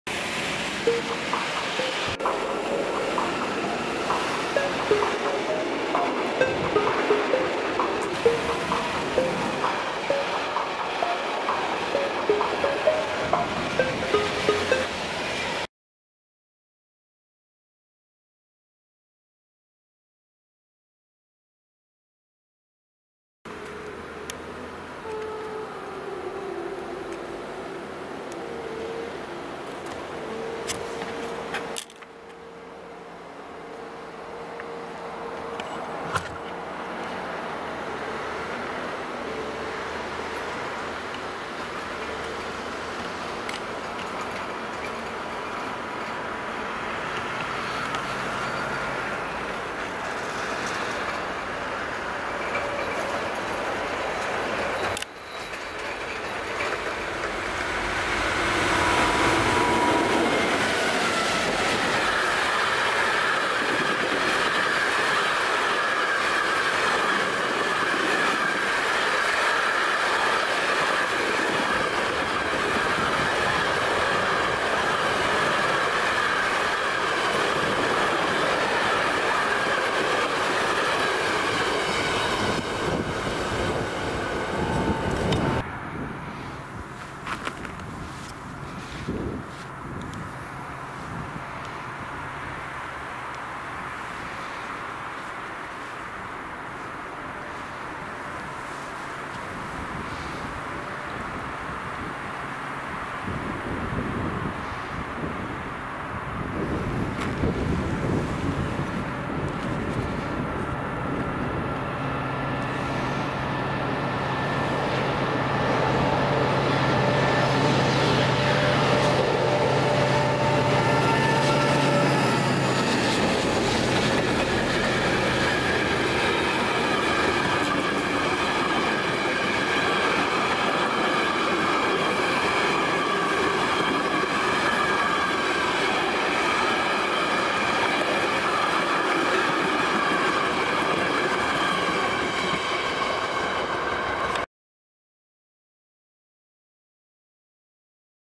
28/11/2023 - Røkland & Oteråga
28/11/2023 To klipp fra Røkland og Oteråga stasjon med Stadler Eurodual ED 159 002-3 med havaret TME 1525 med to tog 5781 og 5791 som rute 8063 (etter fikk stor forsinket) fra Trondheim til Bodø. Velkommen til ser på.

0:00 intro

0:25 Stadler Eurodual ED 159 002 med TME 1525 med tog 8063 (rute 5791 og 5781 koblet sammen) fra Trondheim til Bodø, passerer Røkland holdeplass.

1:26 tog passerer Oteråga stasjon mellom Fauske og Bodø